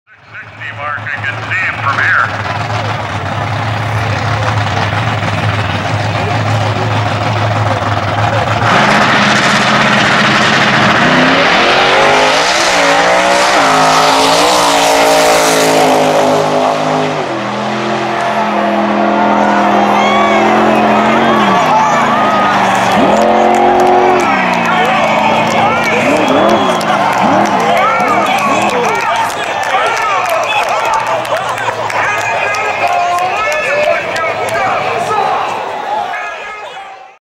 Airborne Cuda Goes 1100 ft on the bumper!! 2014 Byron Wheelstand Contest winner
AJ takes his airborne cuda 1100 ft down Byron Dragways strip on the back bumper